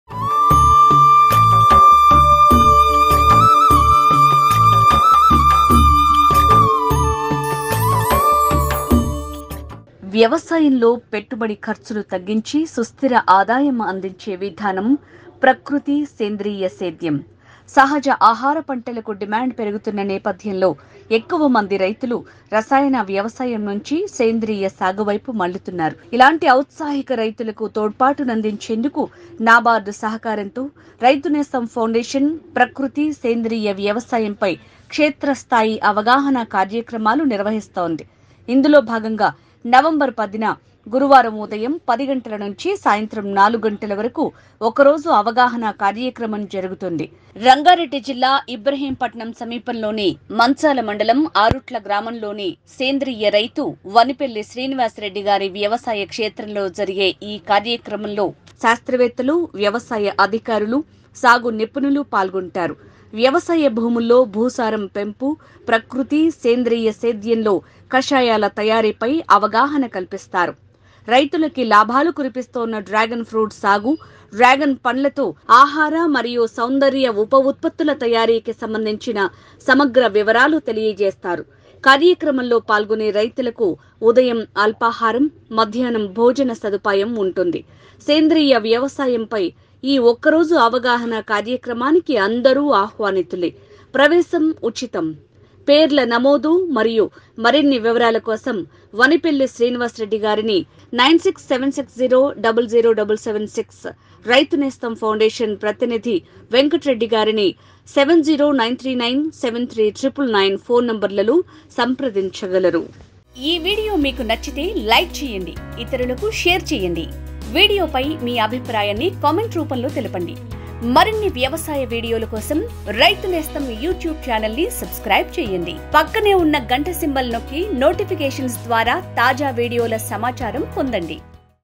డ్రాగన్ ఫ్రూట్ ఎలా పండిస్తారు ? మార్కెటింగ్, ఉప ఉత్పత్తులు ఎలా ? Dragon Fruit Farming & by-products
వ్యవసాయంలో పెట్టుబడి ఖర్చులు తగ్గించి, సుస్థిర ఆదాయం అందించే విధానం... ప్రకృతి - సేంద్రియ సేద్యం. సహజ ఆహార పంటలకు డిమాండ్ పెరుగుతున్న నేపథ్యంలో... ఎక్కువ మంది రైతులు రసాయన వ్యవసాయం నుంచి సేంద్రియ సాగు వైపు మళ్లుతున్నారు. ఇలాంటి ఔత్సాహిక రైతులకి తోడ్పాటు అందించేందుకు నాబార్డ్ సహకారంతో రైతునేస్తం ఫౌండేషన్. ప్రకృతి - సేంద్రియ వ్యవసాయంపై క్షేత్రస్థాయి అవగాహన కార్యక్రమాలు నిర్వహిస్తోంది. ఇందులో భాగంగా... నవంబర్ 10న గురువారం ఉదయం 10 గంటల నుంచి సాయంత్రం 4 గంటల వరకు ఒక రోజు అవగాహన కార్యక్రమం జరుగుతుంది.

రంగారెడ్డి జిల్లా ఇబ్రహీంపట్నం సమీపంలోని మంచాల మండలం ఆరుట్ల గ్రామంలోని సేంద్రియ రైతు వనిపెల్లి శ్రీనివాస్ రెడ్డి గారి వ్యవసాయ క్షేత్రంలో జరిగే ఈ కార్యక్రమంలో... శాస్త్రవేత్తలు, వ్యవసాయ అధికారులు, సాగు నిపుణులు పాల్గొంటారు. వ్యవసాయ భూముల్లో భూసారం పెంపు, ప్రకృతి, సేంద్రియ సేద్యంలో కషాయాల తయారీపై అవగాహన కల్పిస్తారు. రైతులకి లాభాలు కురిపిస్తోన్న డ్రాగన్ ఫ్రూట్ సాగు. డ్రాగన్ పండ్లతో ఆహార మరియు సౌందర్య ఉప ఉత్పత్తుల తయారీకి సంబంధించిన సమగ్ర వివరాలు తెలియజేస్తారు. కార్యక్రమంలో పాల్గొనే రైతులకి ఉదయం అల్పాహారం, మధ్యాహ్నం భోజన సదుపాయం ఉంటుంది. సేంద్రియ వ్యవసాయంపై ఈ ఒక రోజు అవగాహన కార్యక్రమానికి అందరూ ఆహ్వానితులే. ప్రవేశం ఉచితం. పేర్ల నమోదు మరియు మరిన్ని వివరాల కోసం వనిపెల్లి శ్రీనివాస్ రెడ్డి గారిని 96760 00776. రైతునేస్తం ఫౌండేషన్ ప్రతినిధి వెంకట రెడ్డి గారిని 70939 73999 ఫోన్ నంబర్ లో సంప్రదించగలరు.